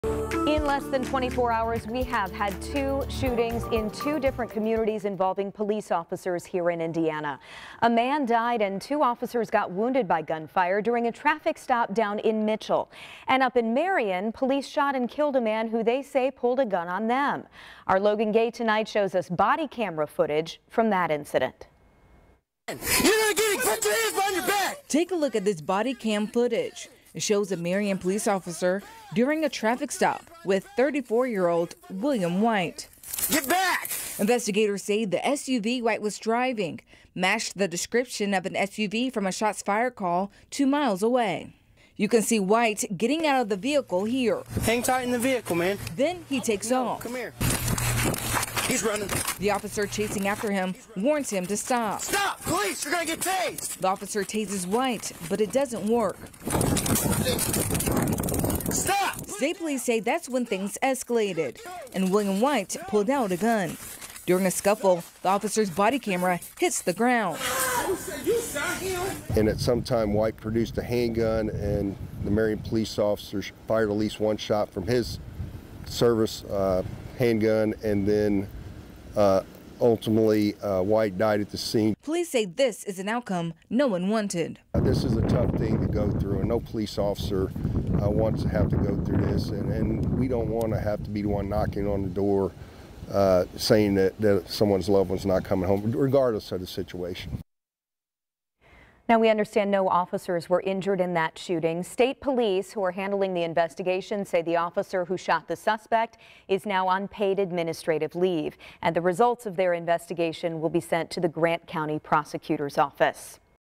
Body camera video shows how incident escalated to deadly officer-involved shooting in Marion
Police claim the suspect, 34-year-old William White, was armed, ignored the officer's commands and reached for the officer's gun before the officer shot him.